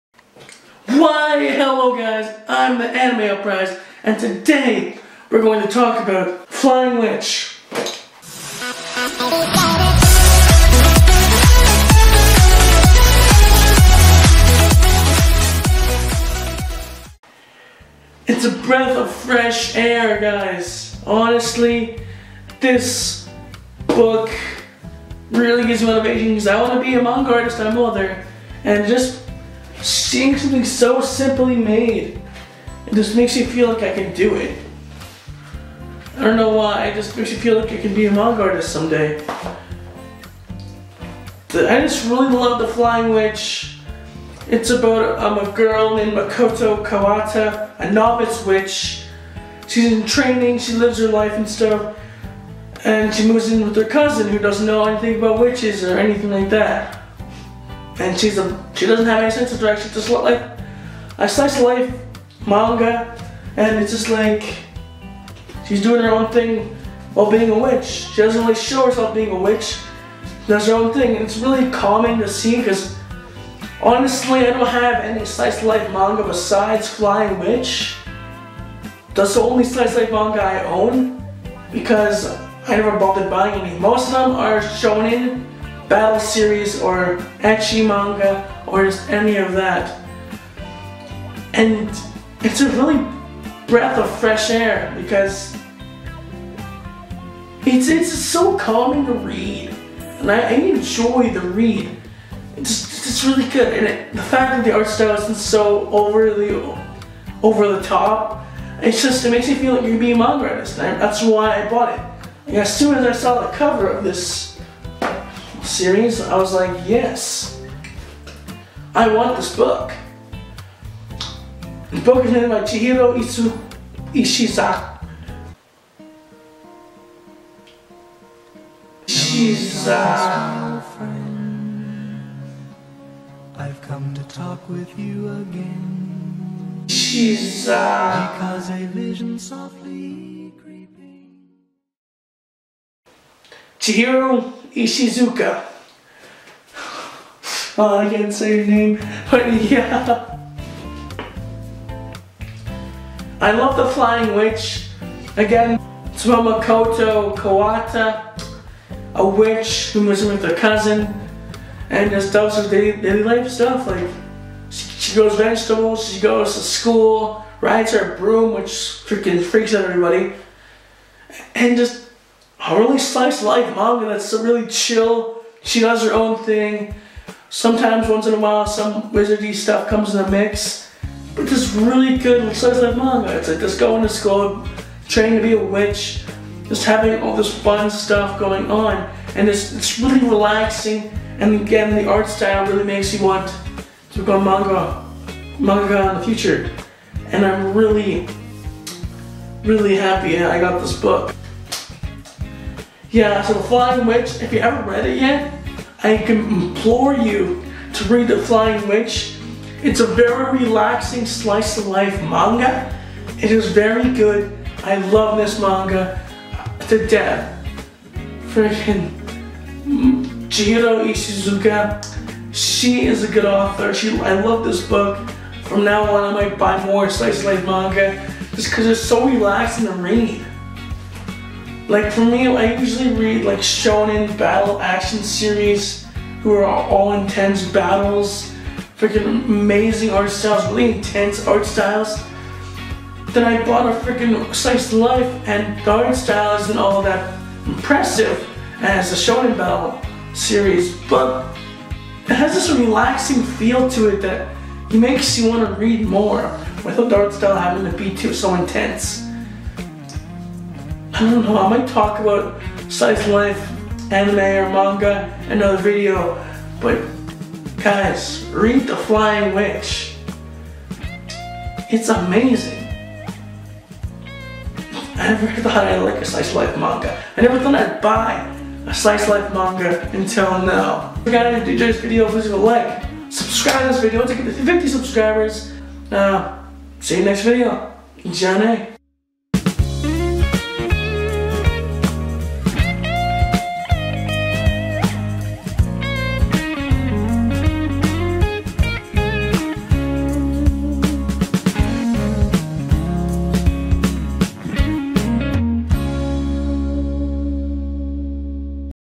(The Flying Witch Volume 1 Review) My First Slice Of Life Manga Series
Hello guys in todays video I'm going to be talking about the Flying witch and how it's my first slice of life manga series and ill also talk about how much i like the book too but yeah if you enjoyed then leave a like and if you really enjoyed Consider subscribing but anyways guys ill see you in the next video